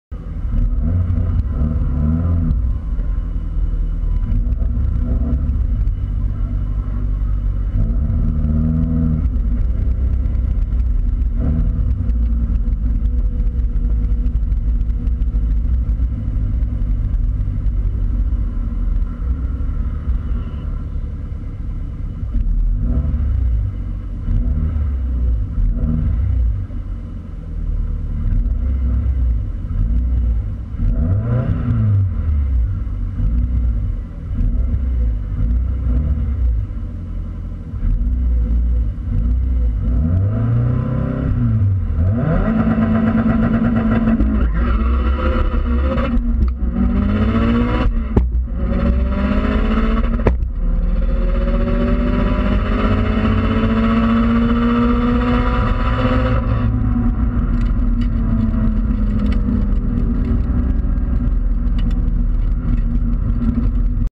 04 STI 11 5 at 122mph Byron Dragway 6 1 2014
Better launch, need to roll out the gears a bit more.  Car is faster than driver still...